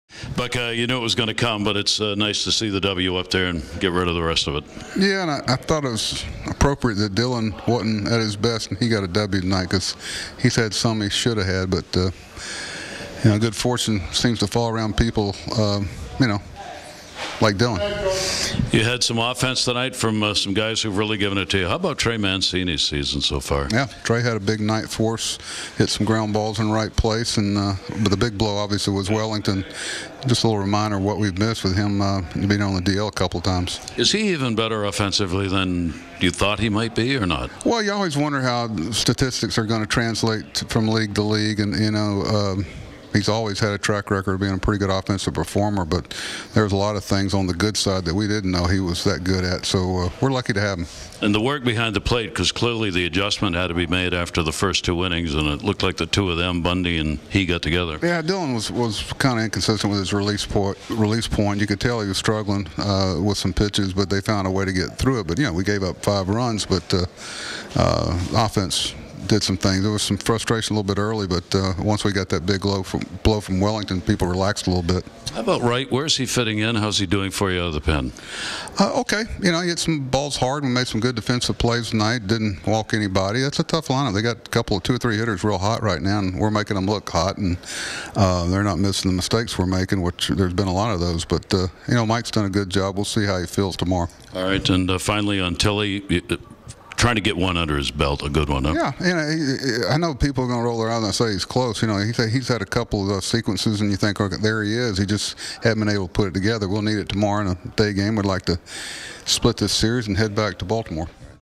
Buck Showalter on the Orioles' win
Buck Showalter discusses the Orioles' 10-6 win over the White Sox with Gary Thorne. More